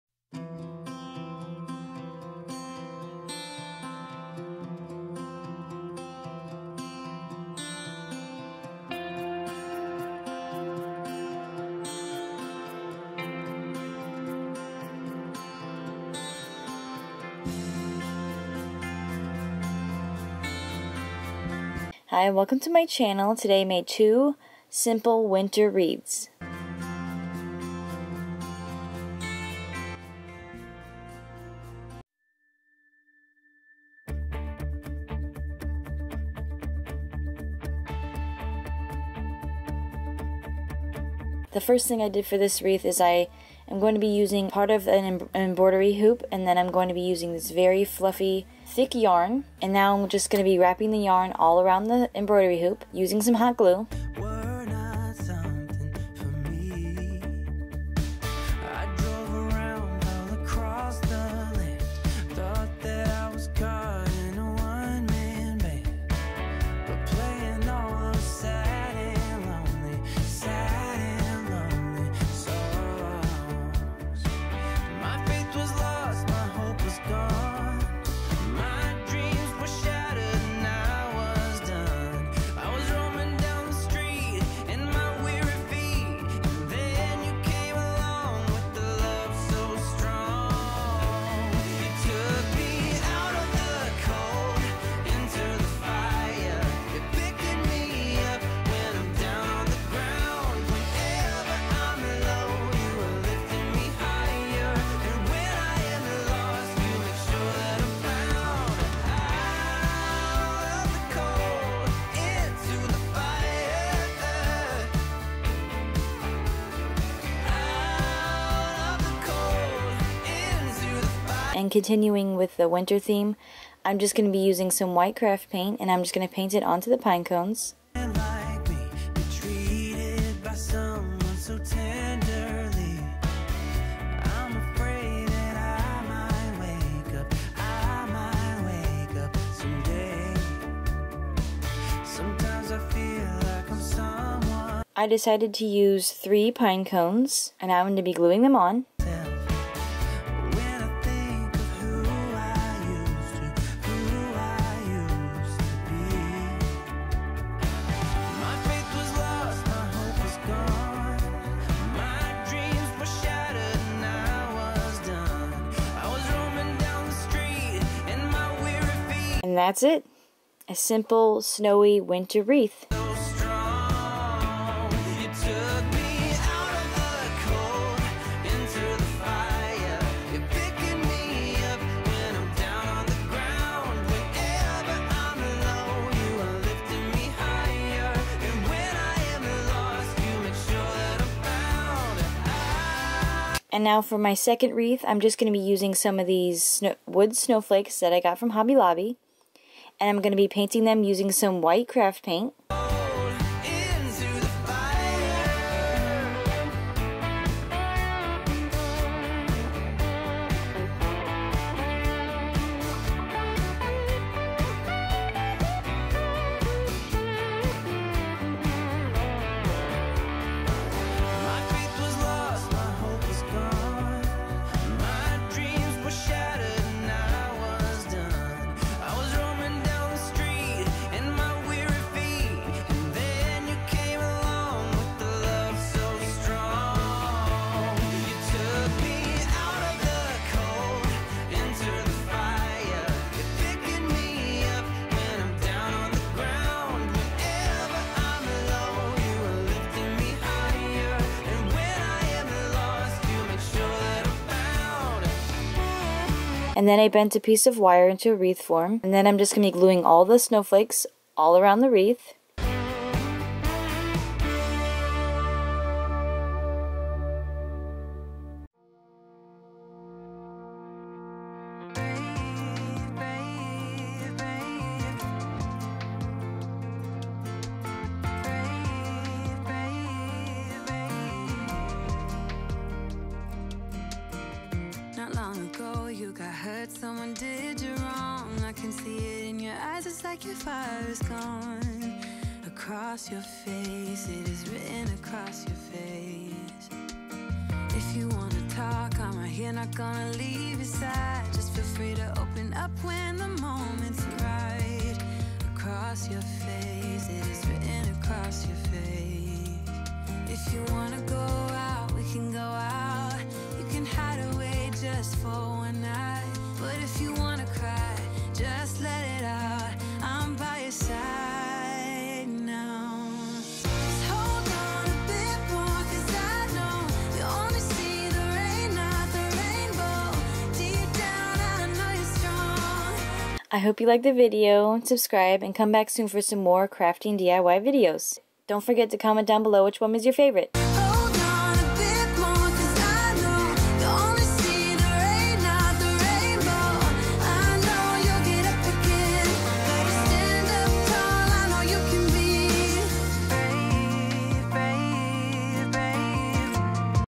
2 SIMPLE AND EASY WINTER WREATHS❄ / SIMPLE WINTER DIYS
2 SIMPLE AND EASY WINTER WREATHS❄ / SIMPLE WINTER DIYS

In todays video I made 2 simple winter DIYs!❄